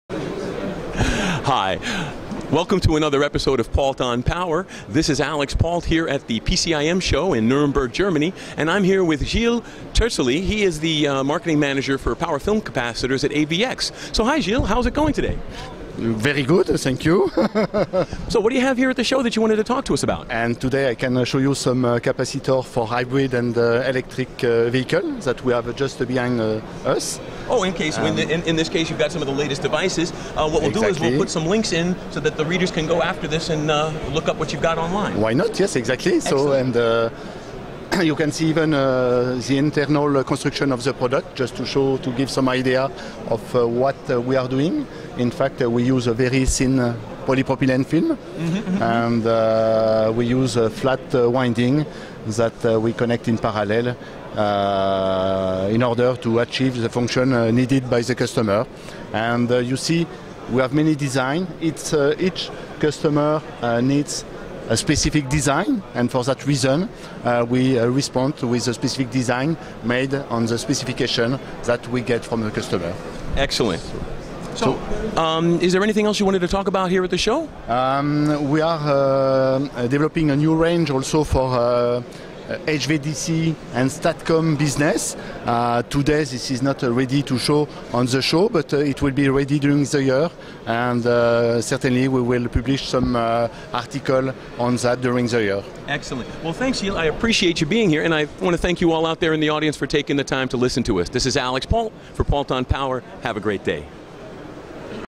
AVX discusses their latest hybrid-vehicle capacitor technology at PCIM 2013
Gilles Terzulli of AVX talks to Alix Paultre of Power Systems Design about the company's latest capacitor range for hybrid and electric vehicles.